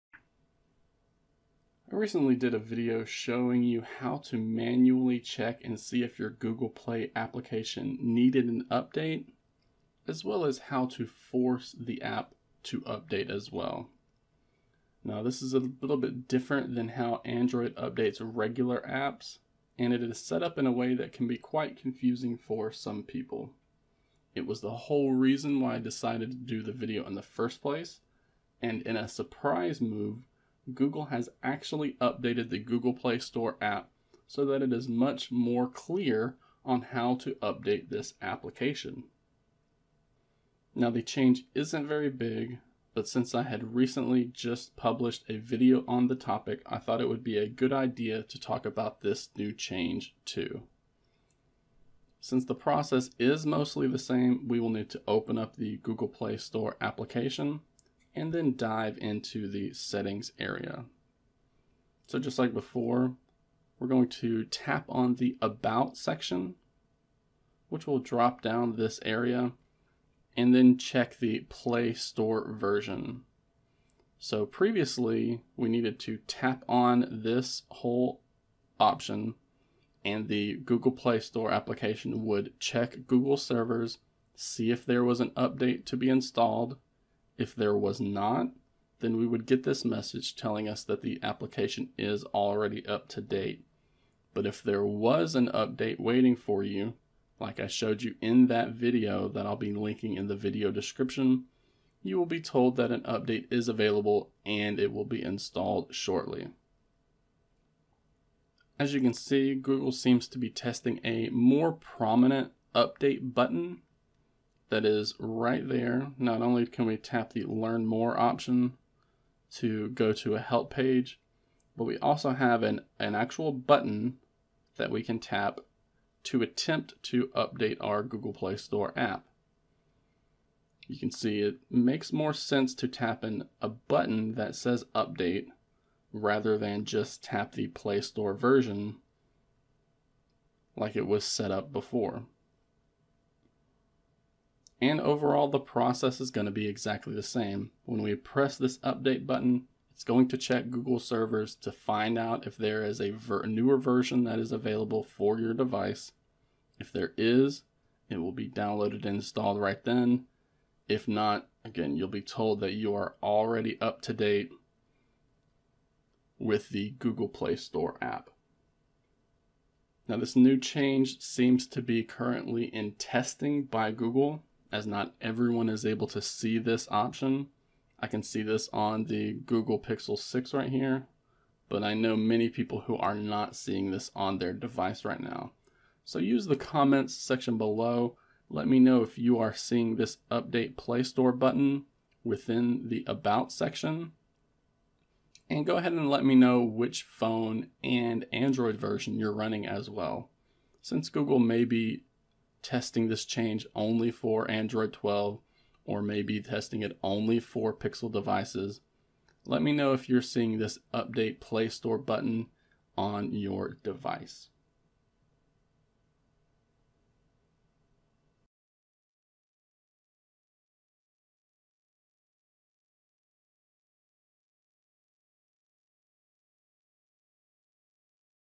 Google Finally Adds a Button to Update the Play Store Application
Google seems to be testing a new way to manually update the Play Store application.

Video Description
A couple of weeks ago I did a video showing you how to manually update the Google Play Store application. I did this because the process was rather obtuse and not many people were aware that it was possible.

Since then, the company has gone through the process to add a button to the About page of the Play Store app.

This makes it much more clear on how to actually install the latest update to Google Play. Even though the placement and design seems a bit sloppy, at least the new button conveys exactly how to update the application.

Full Tutorial
1. Intro [00:00]
2.

The Gear I Use:
5W Amazon Charger